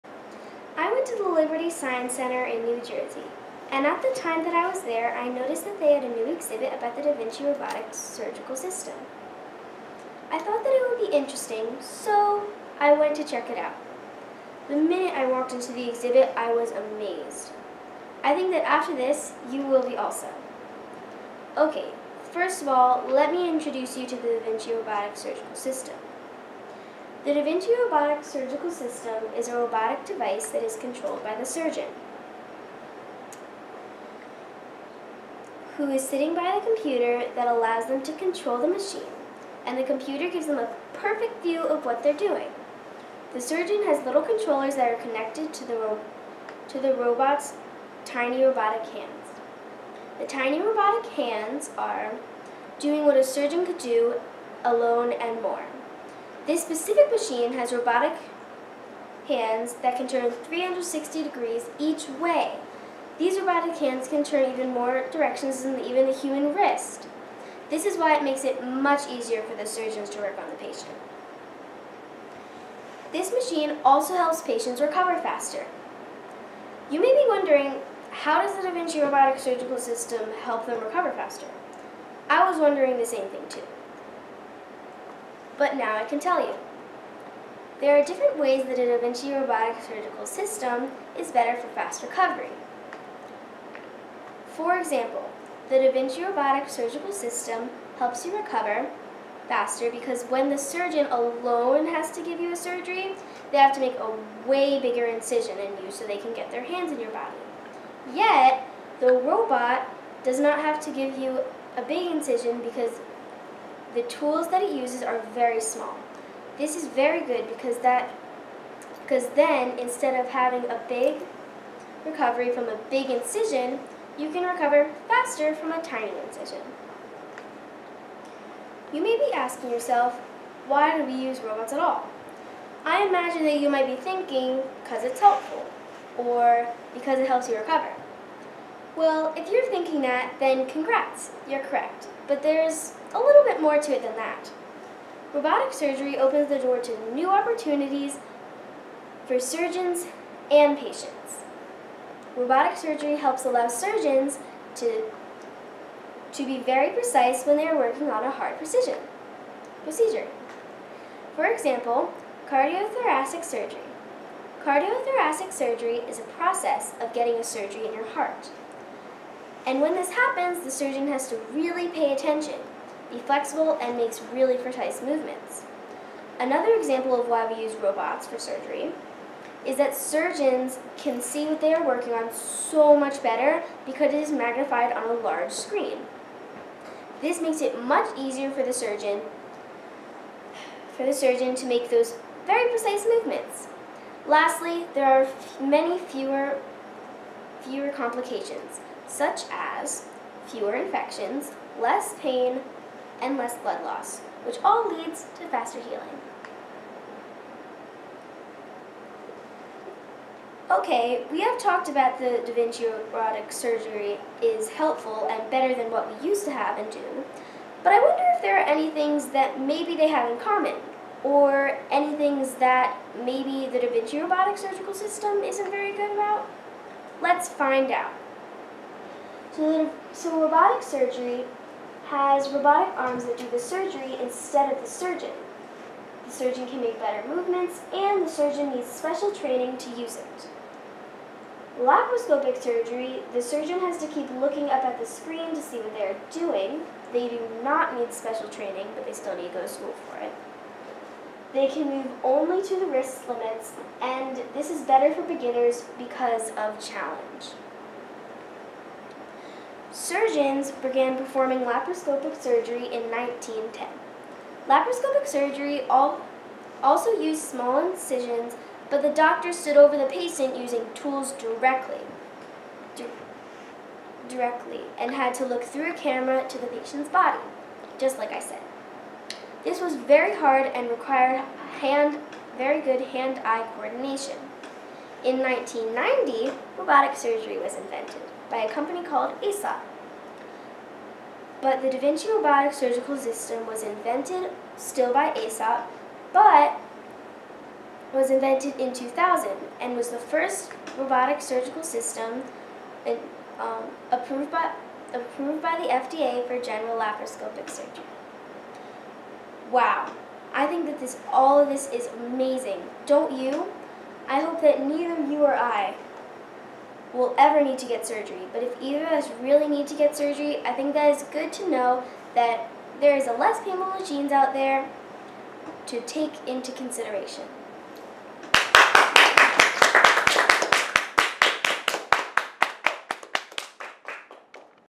The Da Vinci Robotic Surgery | Julia Coppo | Wyckoff Schools
The main reason that Julia chose Da Vinci Robotic Surgery is because of the opportunities it has afforded people around the world.  She is excited to watch as robotics helps to transform the ways we can help others.

Julia is curious. Her many interests like include science, robots, the Earth and things that will make a difference. This is one of the reasons that she chose to feature the Da Vinci Robotic Surgery for her Ted Ed talk this year. This system has opened so many doors of opportunity for surgeons and patients around the globe. She hopes the future will bring more new machines and robots that can help people that have disabilities and people who have wheelchairs to live the life they have always wanted to live. When she grows up her goal is to help create and discover these systems.